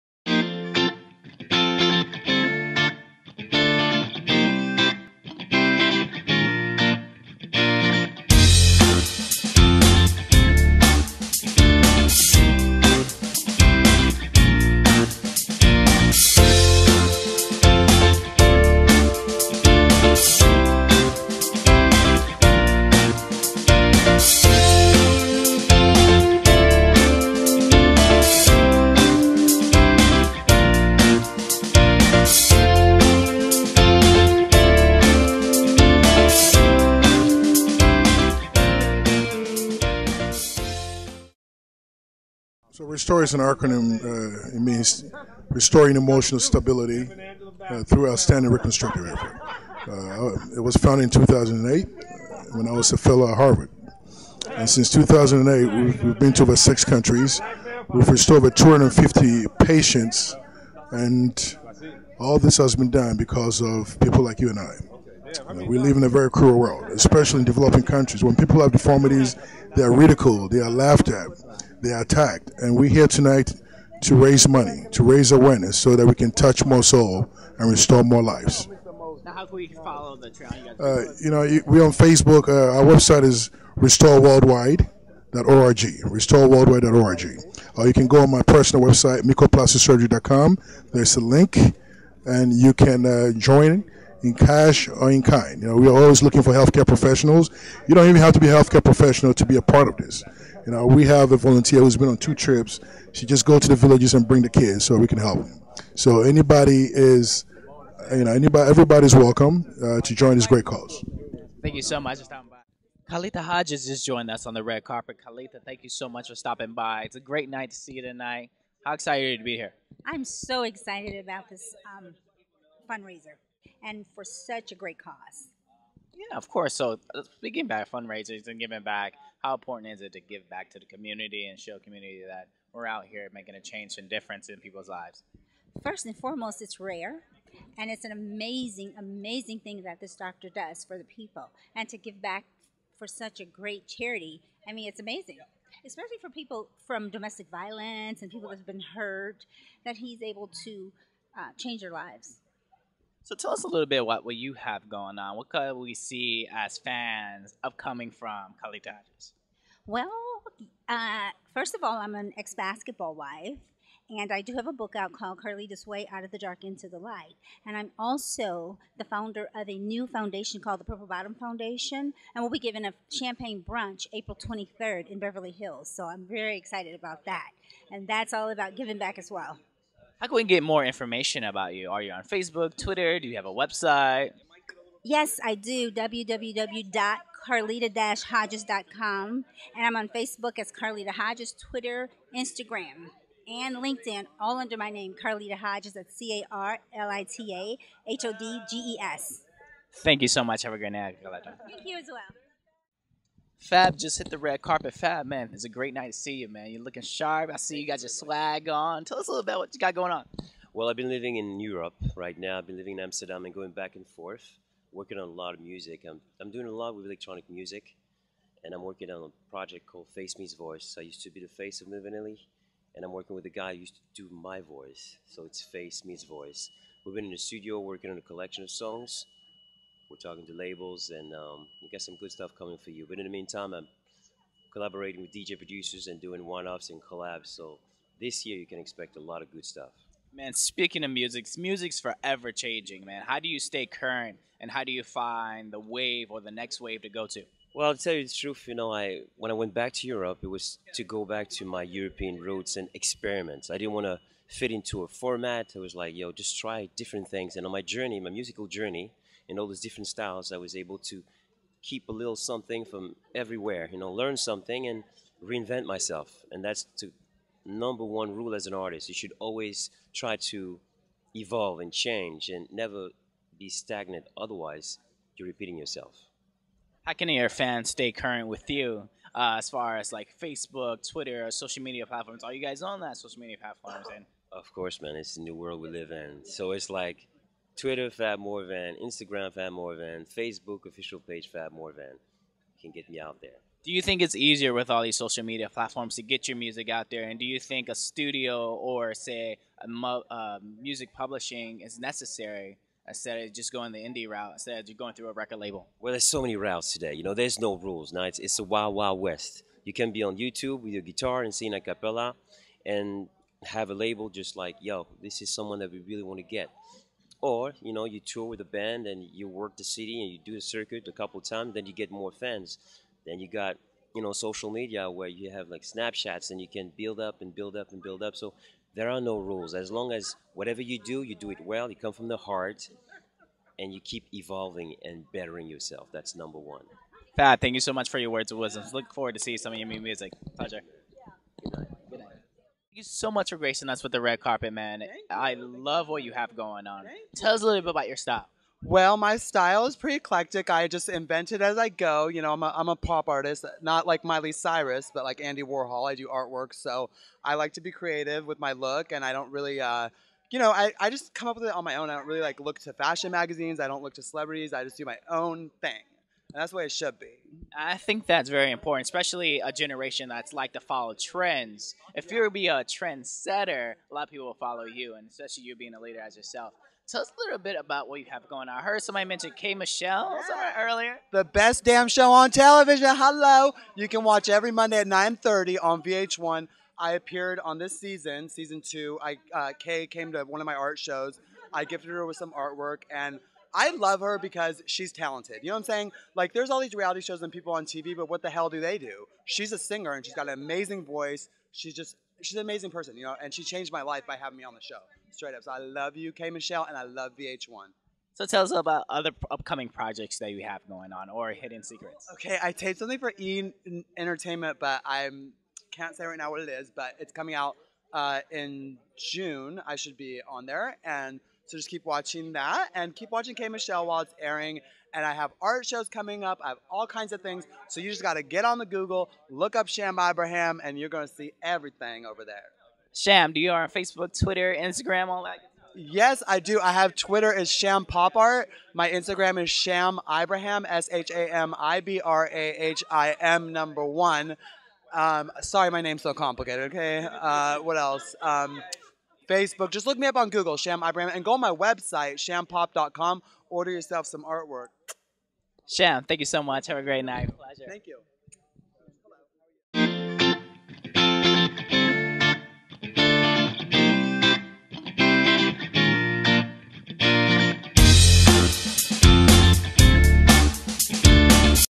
Dr. Michael K. OBeng Charity Gala Birthday Party for R.E.S.T.O.R.E
Celebrities and VIPS attend Dr. Michael K. OBeng Charity birthday gala for his non profit organization R.E.S.T.O.R.E